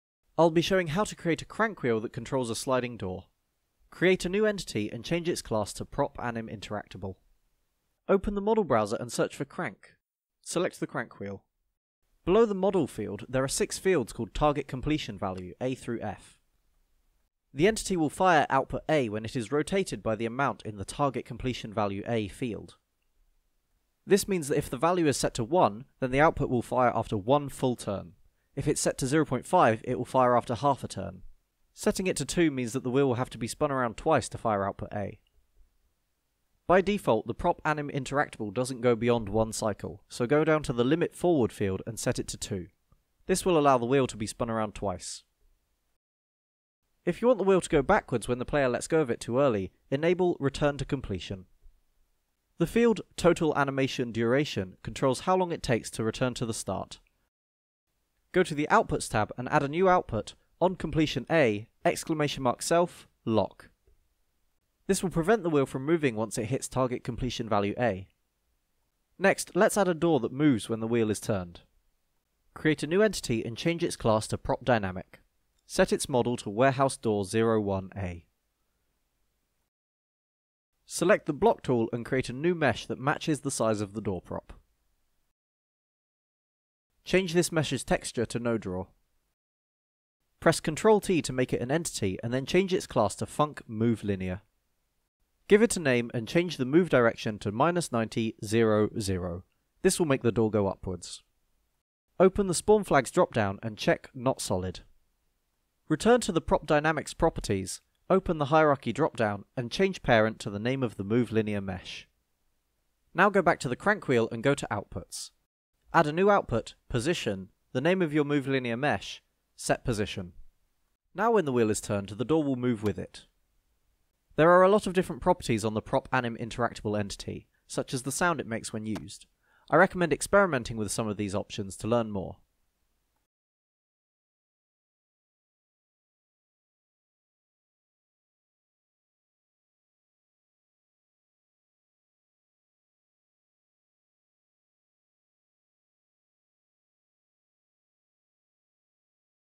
How To Create a Crank Wheel and Shutter Door in Source 2 Hammer
This tutorial involves the use of the prop_animinteractable entity to create a crank wheel that controls a func_movelinear and a prop_dynamic rolling shutter door.
This can be used to create all sorts of dynamic systems for the player to interact with.

Made using the Half Life Alyx Workshop Tools version of Hammer for Source 2